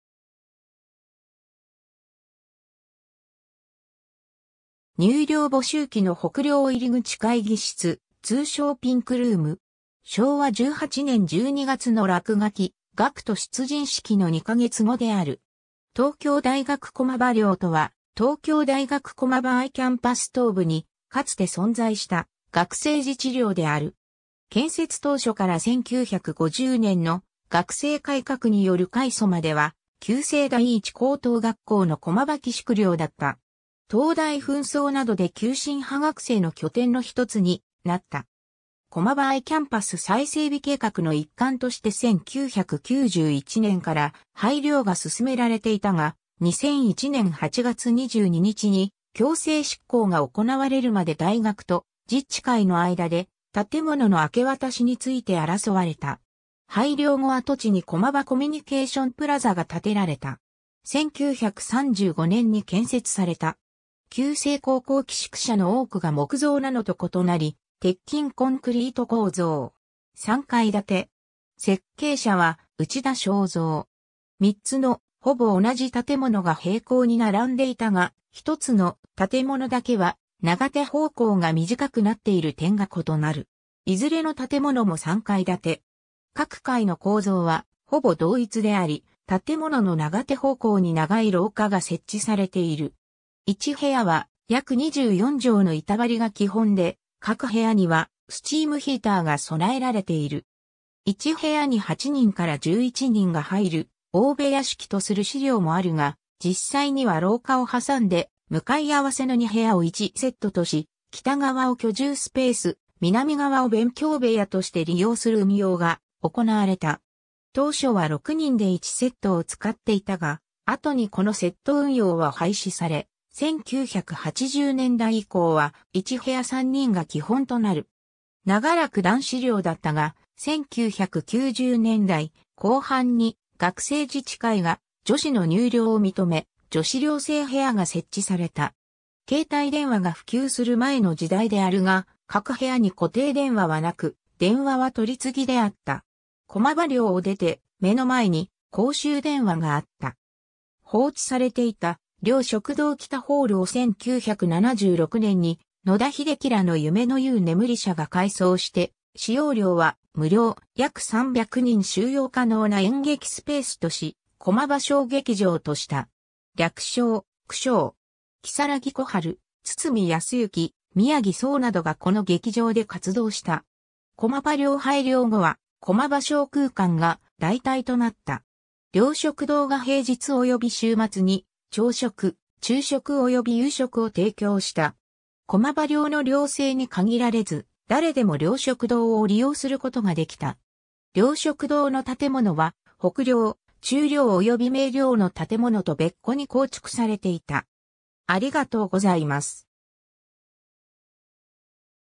東京大学駒場寮
入寮募集期の北寮入口 会議室、通称ピンクルーム。
 昭和18年12月の落書、学徒出陣式の2ヶ月後である。
 東京大学 駒場寮（とうきょうだいがく こまばりょう）とは、東京大学駒場Iキャンパス東部にかつて存在した学生自治寮である。
建設当初から1950年の学制改革による改組までは旧制第一高等学校の駒場寄宿寮だった。
東大紛争などで急進派学生の拠点の一つになった。
 駒場Iキャンパス再整備計画の一環として1991年から廃寮が進められていたが、2001年8月22日に強制執行が行われるまで大学と自治会の間で建物の明け渡しについて争われた。
廃寮後跡地に駒場コミュニケーション・プラザが建てられた。
 1935年に建設された。
旧制高校寄宿舎の多くが木造なのと異なり鉄筋コンクリート構造。
3階建て。
設計者は内田祥三。
 3つのほぼ同じ建物（北寮、中寮及び明寮）が平行に並んでいたが、1つの建物（明寮）だけは長手方向が短くなっている点が異なる。
いずれの建物も３階建て。
各階の構造はほぼ同一であり、建物の長手方向に長い廊下が設置されている。
 1部屋は約24畳の板張りが基本で、各部屋にはスチームヒーターが備えられている。
「1部屋に8人から11人が入る大部屋式」とする資料もあるが、実際には廊下を挟んで向かい合わせの2部屋を1セットとし、北側を居住スペース、南側を勉強部屋として利用する運用が行われた。
当初は6人で1セットを使っていたが、後にこのセット運用は廃止され、1980年代以降は1部屋3人が基本となる。
 長らく男子寮だったが、1990年代後半に学生自治会が女子の入寮を認め、女子寮生部屋が設置された。
 携帯電話が普及する前の時代であるが、各部屋に固定電話はなく、電話は取り次ぎであった。
駒場寮を出て、目の前に公衆電話があった。
 放置されていた「寮食堂北ホール」を1976年に野田秀樹らの夢の遊眠社が改装して、使用料は無料、約300人収容可能な演劇スペースとし、「駒場小劇場」とした。
略称「駒小」。
如月小春、堤泰之、宮城聰などがこの劇場で活動した。
駒場寮廃寮後は「駒場小空間」が代替となった。
 寮食堂が平日及び週末に朝食、昼食及び夕食を提供した。
駒場寮の寮生に限られず、誰でも寮食堂を利用することができた。
 寮食堂の建物は、北寮、中寮及び明寮の建物と別個に構築されていた。